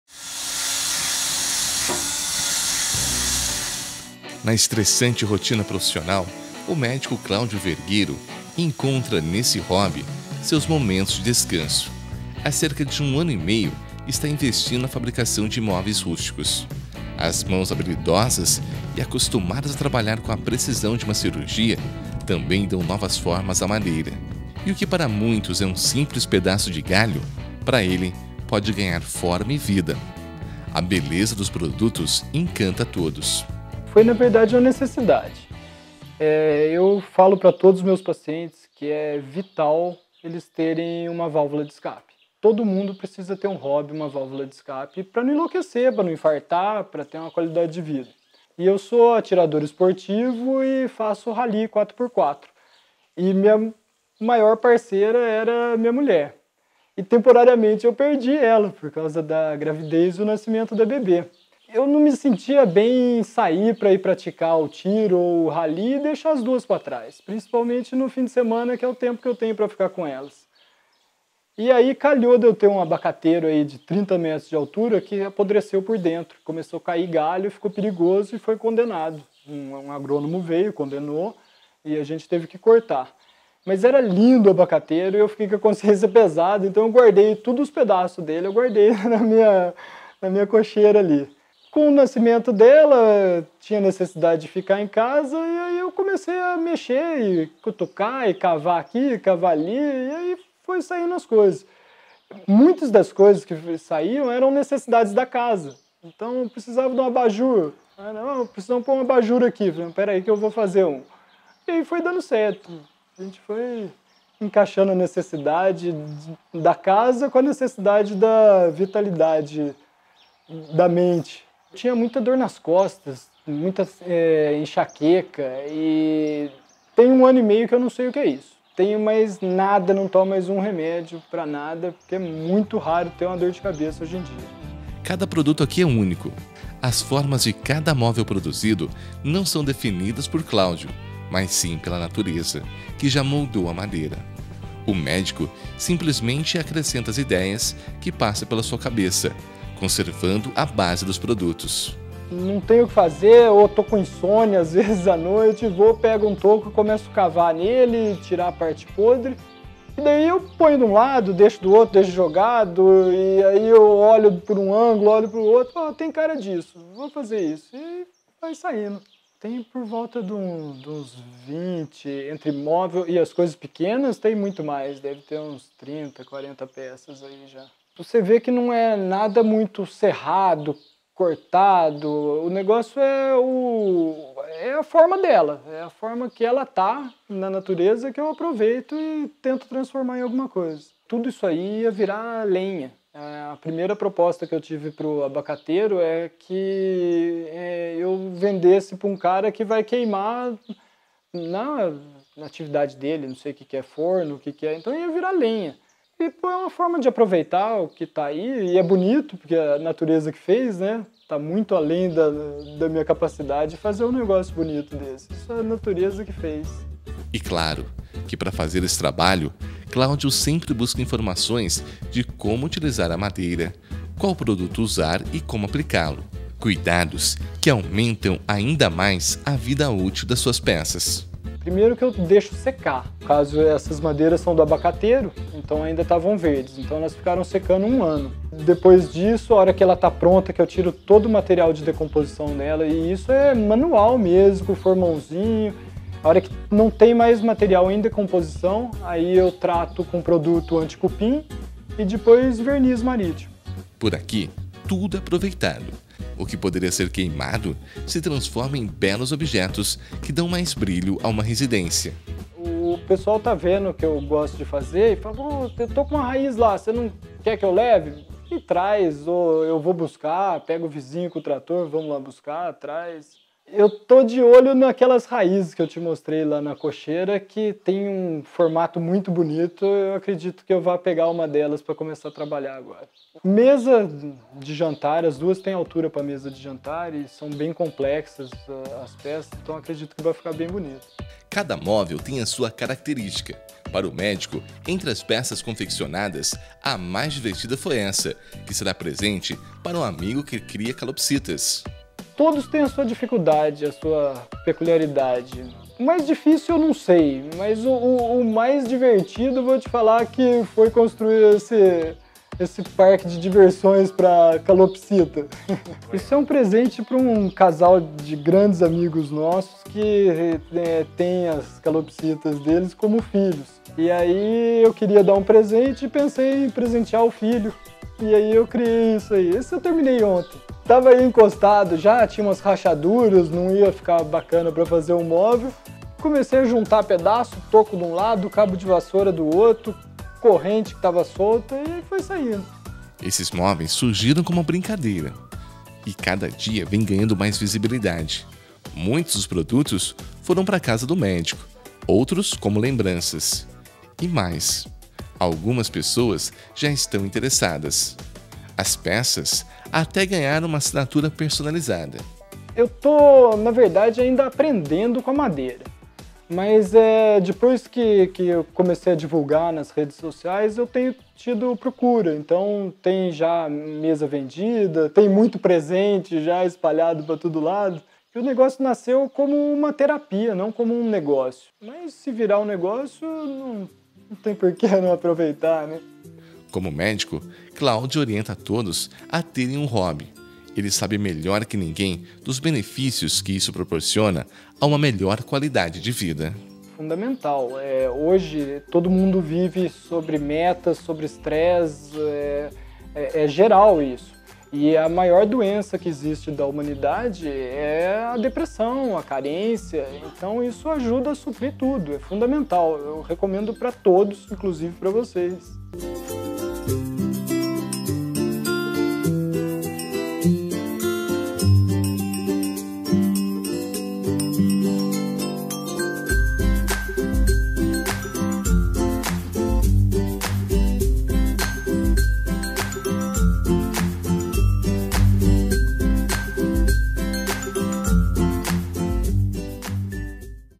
Fabricação de Móveis Rústicos em Andradas
Médico de Andradas utiliza as horas vagas para se dedicar a um hobby. Transforma madeira em móveis rústicos.